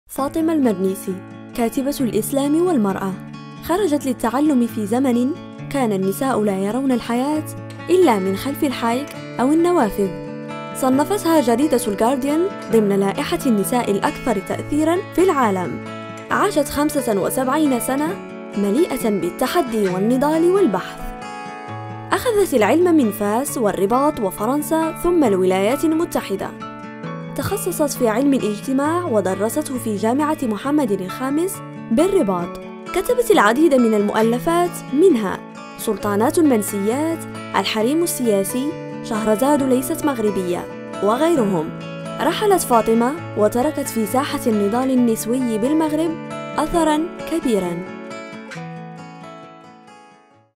فاطمة المرنيسي.. كاتبة الإسلام والمرأة
فاطمة المرنيسي، باحثة وعالمة اجتماع وكاتبة نسوية مغربية، اهتمت بالإسلام والمرأة وتحليل تطور الفكر الإسلامي والتطورات الحديثة.

.من بين كتبها: ما وراء الحجاب، الحريم السياسي، شهرزاد ترحل إلى الغرب، الإسلام والديمقراطية